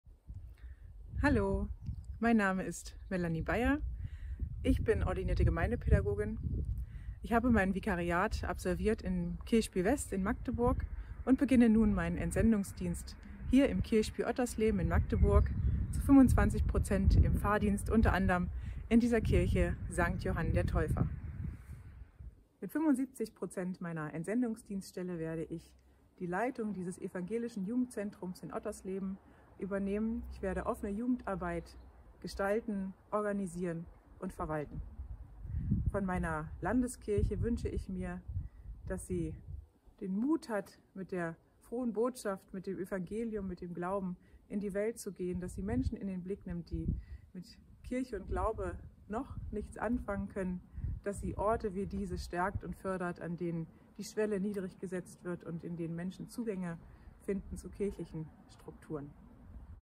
Ordination 2023: Melanie Beyer
Am 7. Mai 2023 werden 20 Frauen und Männer in einem Gottesdienst im Magdeburger Dom ordiniert. Einige von ihnen stellen wir hier vor.